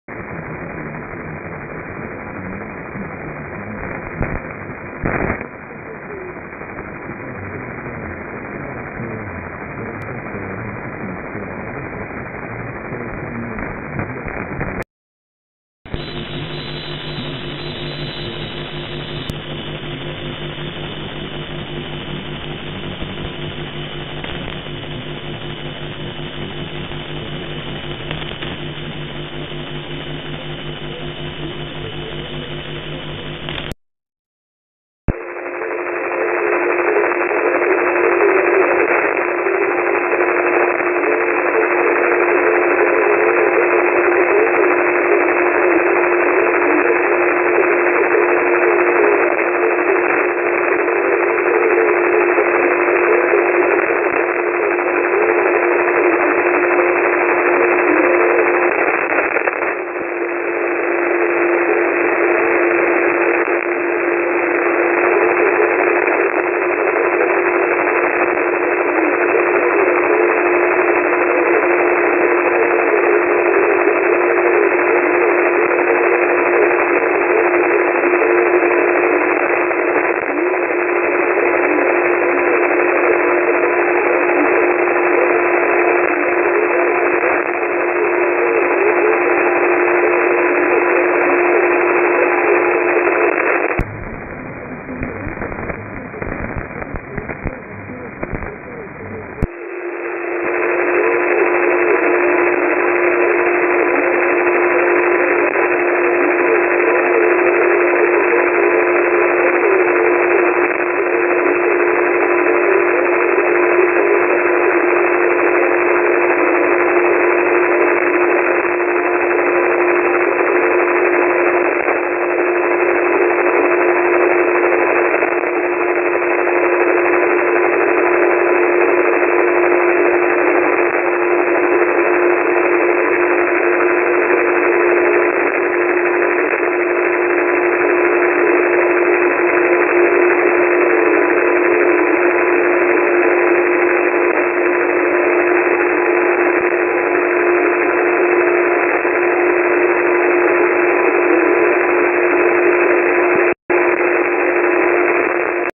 (Almost) Empty Türkmen carrier 279 kHz
Despite good carrier level at my QTH you can hear only some traces of TR1 Watan's program in USB mode.

I hope, they will repair or replace the old soviet transmitter! It could be very exotic DX station for us.

Recorded at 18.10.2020 ~ 2115 UTC